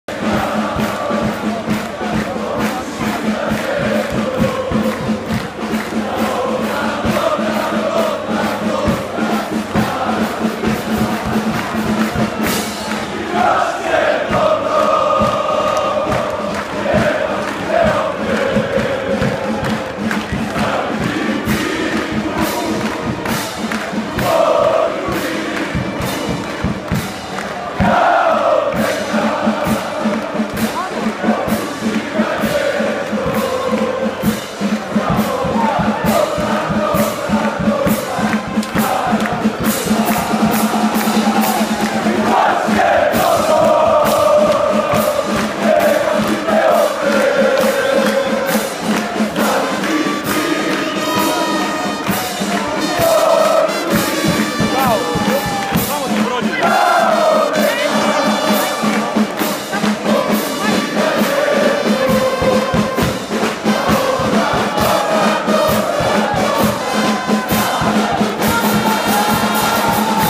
Rujevica eksplodirala: Kekova Rijeka hrvaški prvak!
To se je zgodilo prvič v klubski zgodovini, Kekovi varovanci so naslov potrdili z današnjo zmago s 4:0 nad Cibalio.